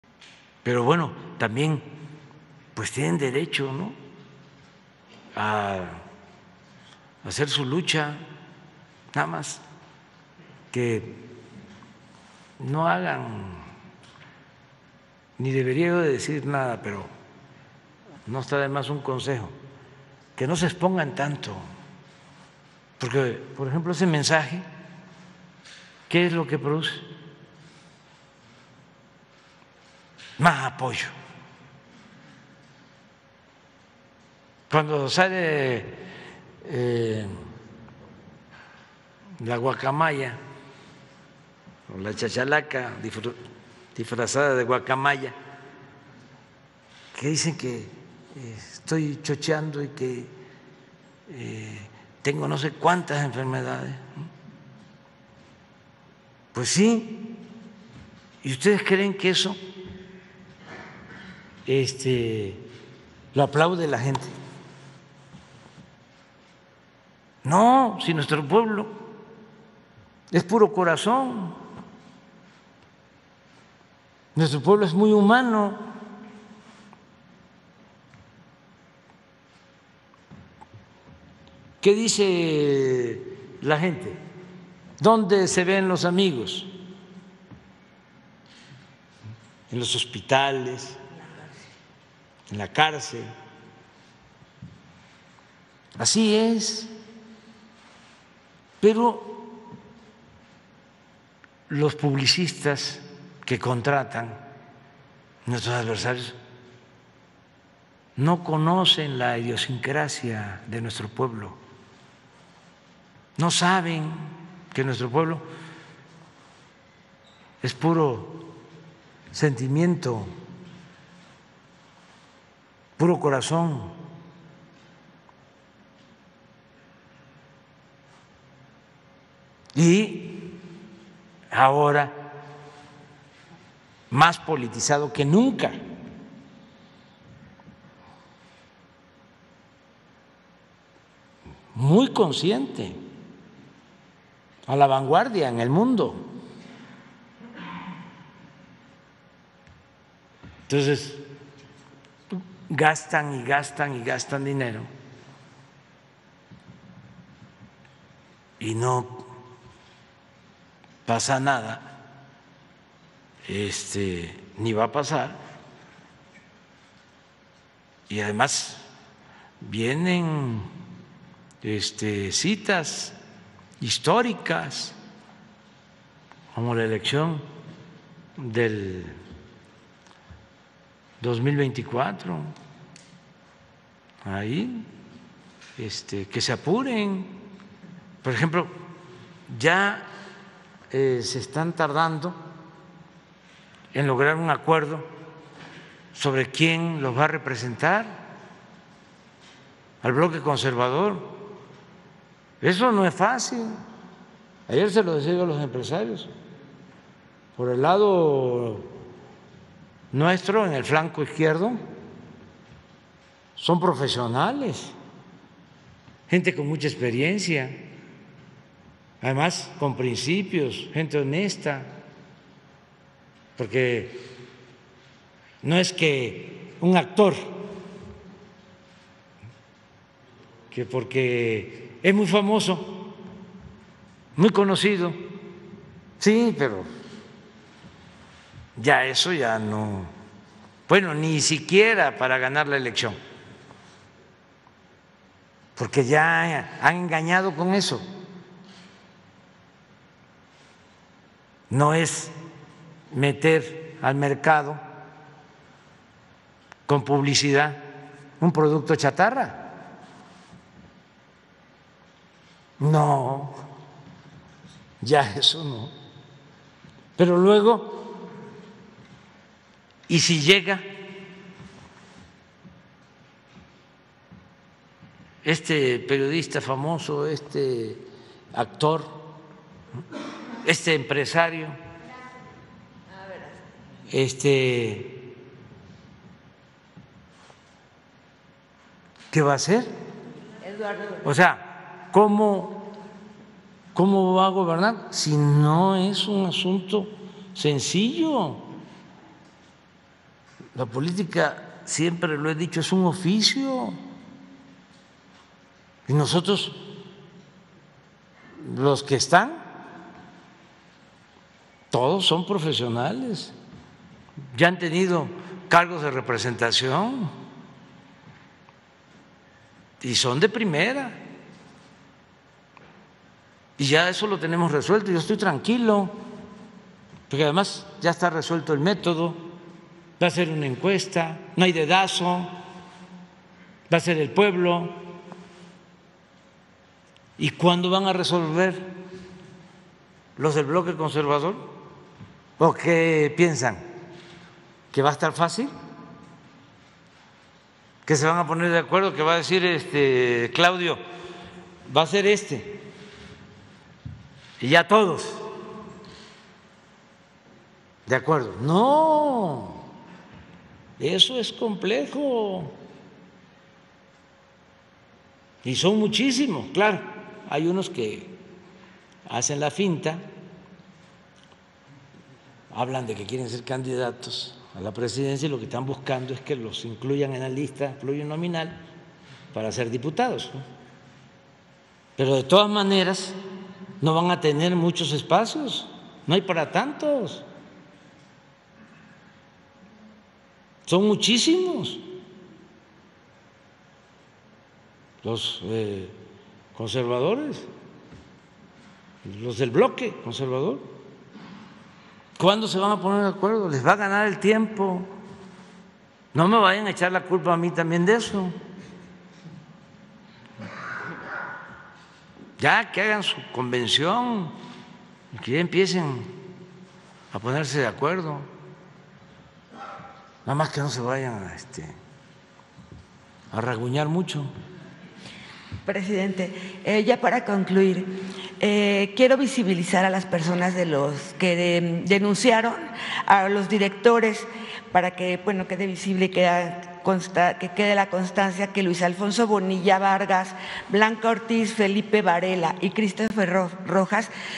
LA LUCHA DEL BLOQUE CONSERVADOR CONFERENCIA PRESIDENTE LOPEZ OBRADOR VIERNES 09 DIC 22
Este fragmento corresponde a la Conferencia Mañanera del Presidente Andrés Manuel Lopez Obrador AMLO de Fecha: VIERNES 09 de Diciembre de 2022